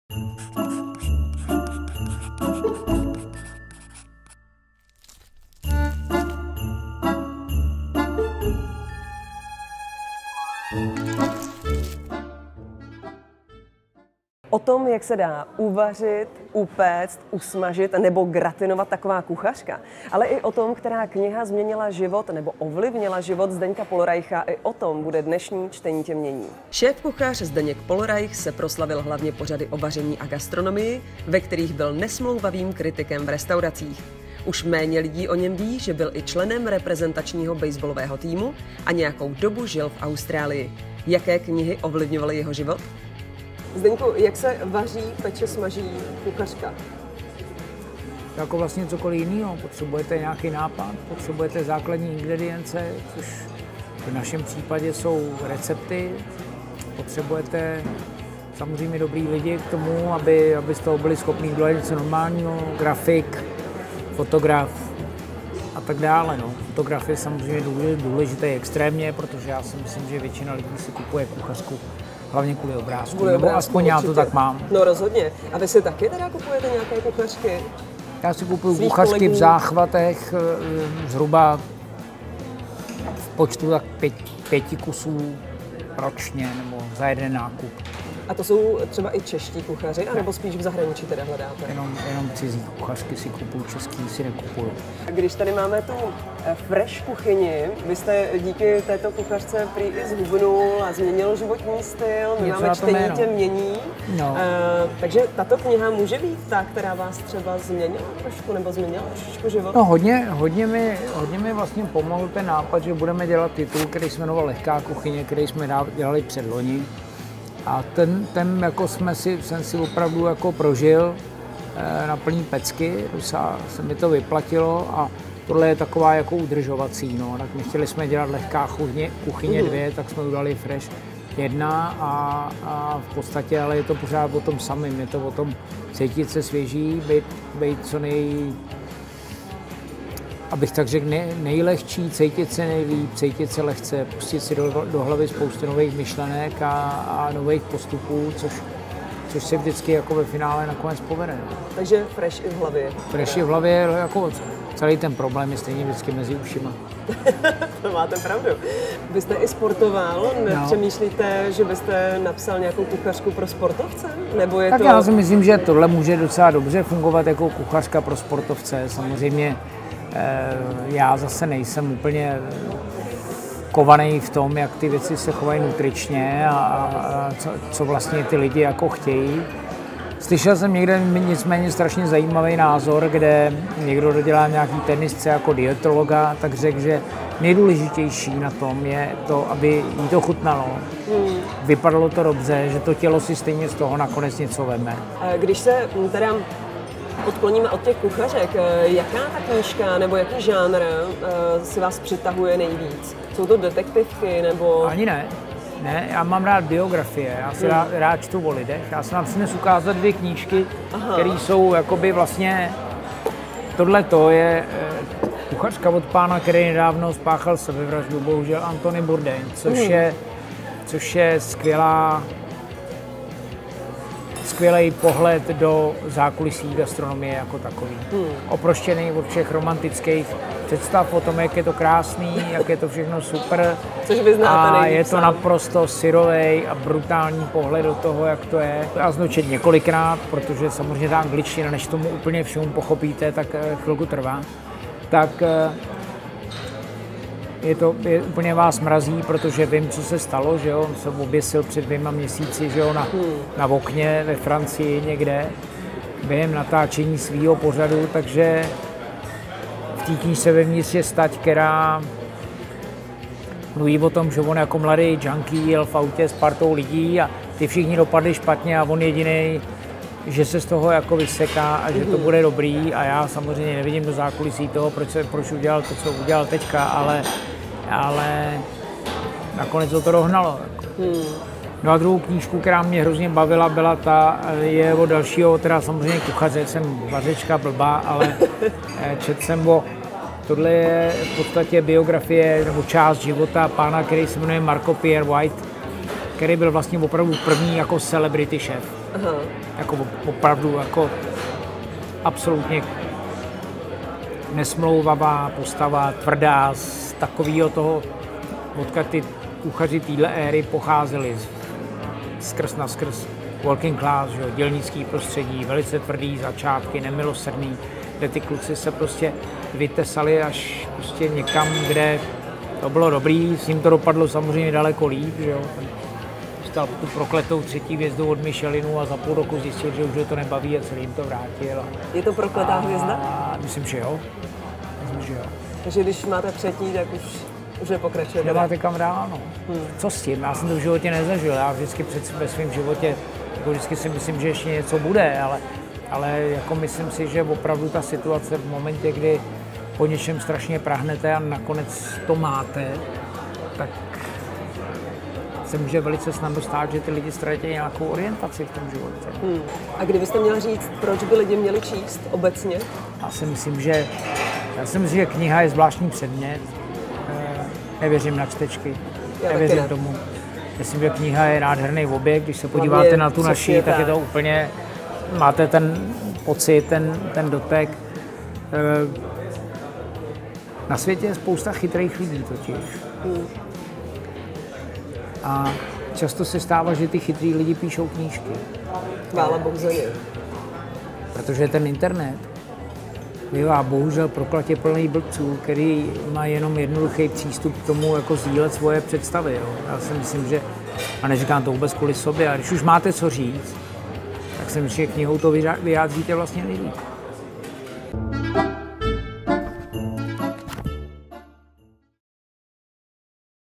ČTENÍ TĚ MĚNÍ: Zdeněk Pohlreich
Zdeněk Pohlreich se svěřil Čtení tě mění, jak se připravuje kuchařka, proč je jeho nová kuchařka Fresh a jaké příběhy rád čte. Nechte se pozvat rovnou do kuchyně! Jako vždy se ptala Lenny Trčková.